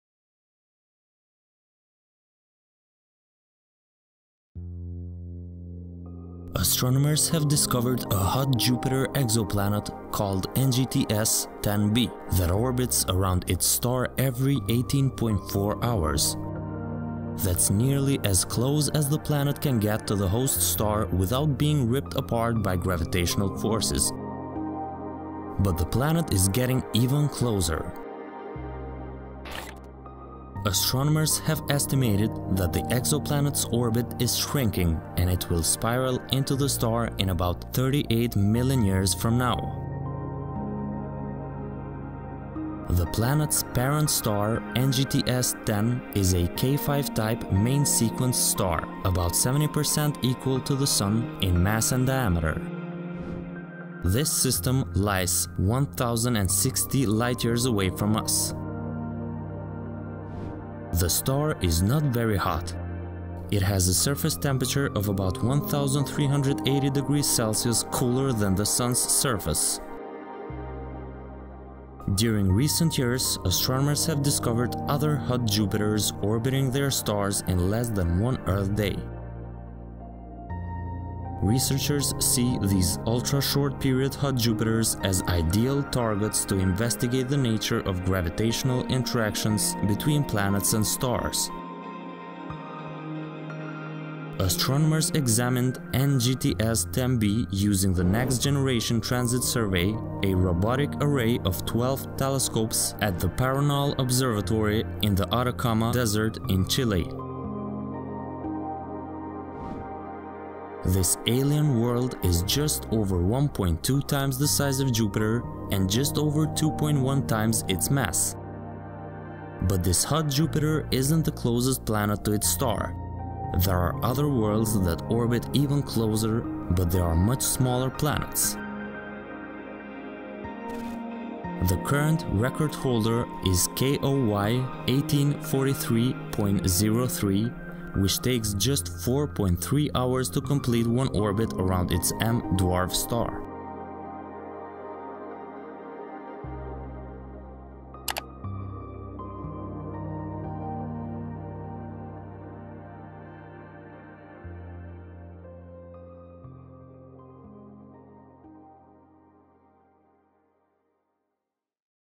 Extreme Hot Jupiter Orbits Every 18 Hours
Astronomers have discovered a hot Jupiter exoplanet, called NGTS-10b, with the shortest orbit yet known. A year on that planet lasts only 18.4 hours.

Graphics & Credits:
Space Engine
Storyblocks.com